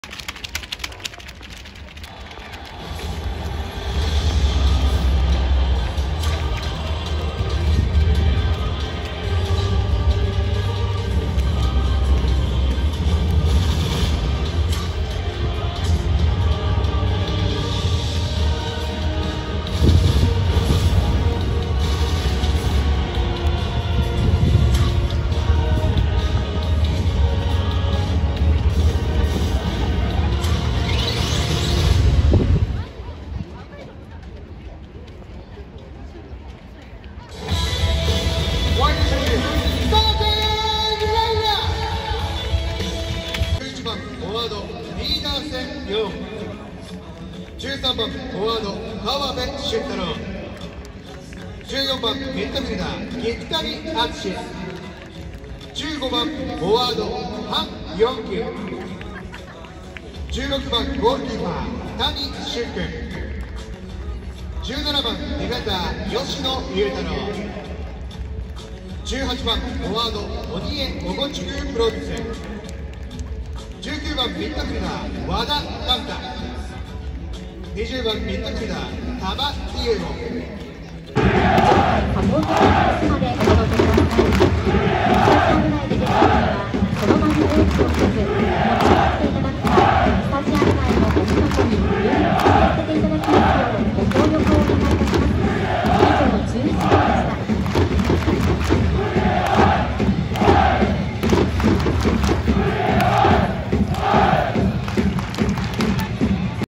2022年J3リーグ YSCC横浜(YS横浜)選手出場記録 Vol.2 オープニングムービー→背番号11番〜20番
〜Ver.487/10JAN23〜

小さい頃から観ていたJ1 横浜FCと湘南ベルマーレ、そしてJ2を目指して強くなっていくJ3 Y.S.C.C横浜を応援しています。

試合前にスタジアムで流れるオープニングムービーや選手紹介は各チーム様々な工夫やオリジナリティがあり、とても面白いです。

2022年J3リーグ YSCC横浜(YS横浜)選手出場記録 Vol.1 オープニングムービー→背番号11番〜20番になります。

一部の選手は動画がないため、割愛している場合があります。ご了承ください。